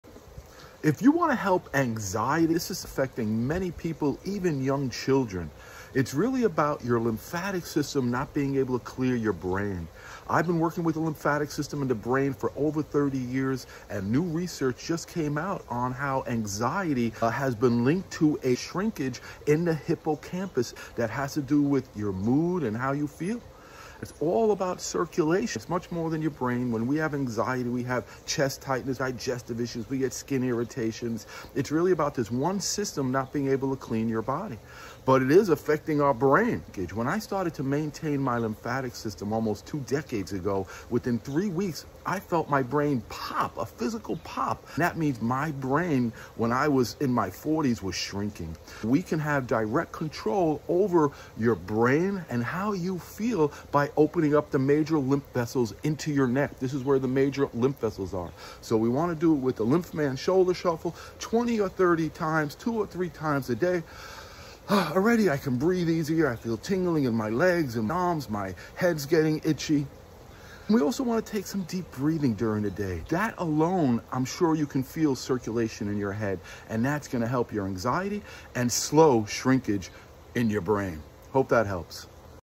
Is Your Brain Making You Anxious?
Is your brain making you anxious? Yes, and anxiety has been linked to shrinkage in the brain.
🧠 How to Reduce Anxiety by Boosting Your Brain's Detox System
Struggling with anxiety—even in young children? It's not just "in your head"... it's in your lymphatic system.

💥 New research links anxiety to shrinkage in the hippocampus—the area of your brain that controls mood and memory. The problem? Your lymphatic system may not be clearing waste out of your brain properly.

Here’s what anxiety really feels like:
- Racing thoughts, poor sleep, brain fog
- Chest tightness
- Skin breakouts or rashes
- Digestive issues

👉 These are signs your brain is overwhelmed with waste and inflammation—and your lymphatic system is compromised and not able to complete circulation.

✅ After 30+ years working with the lymphatic system, I know this:
 When you open up lymph flow in the major lymph vessels in the neck, your brain can finally clear out acidic waste.

By being CONSISTENT, you can feel better—and even stop brain shrinkage in its tracks.

Try this daily:
- LymphMan Shoulder Shuffle - 20–30 reps of the , 2–3x a day
- Take 10-20 Deep Breaths a couple of times during the day

After starting to maintain my lymphatic system in a specific manner in my 40s, after three weeks, I felt a “pop” in my brain, and it changed everything.

Let’s help your brain feel that relief, too. 💫

🔗 Tap my profile or Linktree for full details + join me Tuesday night on Zoom to learn how to heal.